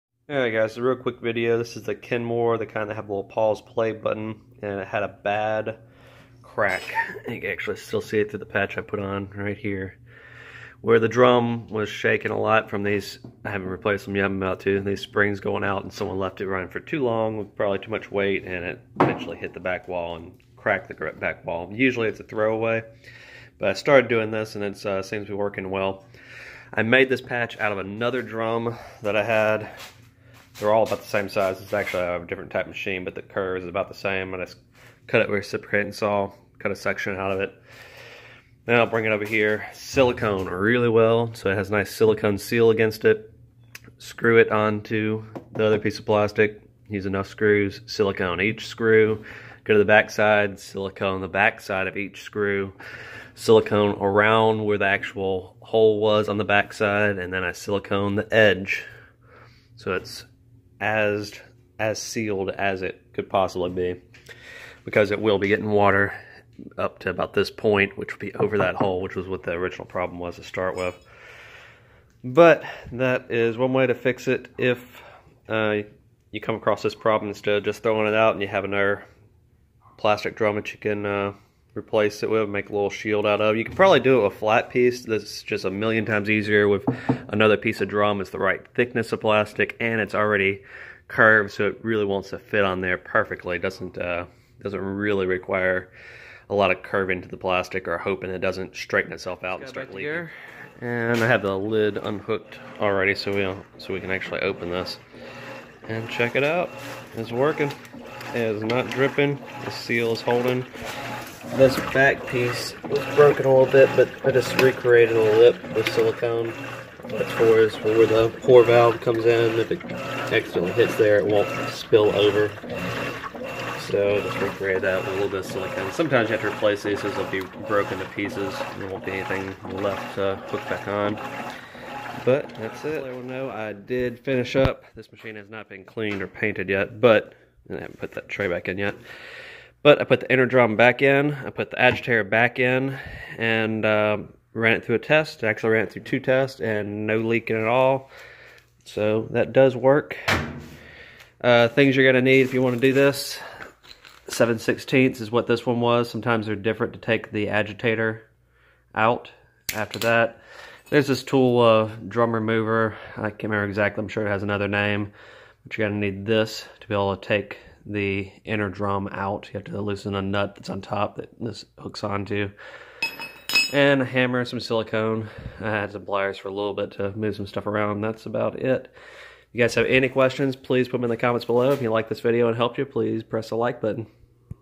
Cracked leaking washer drum tub repair. For Kenmore Whirlpool Maytag and other brands
Sometimes when the suspension rod goes bad the washer will shake and break the plastic￼ tub. This was my solution to fix it.￼

3 easy steps to finding the right part that will “Actually fit”

2. Enter the model number of your machine in the search. (You’ll find Your model number on a sticker inside the door)
3. All the parts that will fit your machine will appear. Just scroll down until you see the part you need.

Common red actuator.

Newer black actuator.

Suspension kit with 4 extra stabilizer springs.

Washer splutch common grinding noise part some of these are a little different you should double check it’s the right part before you order.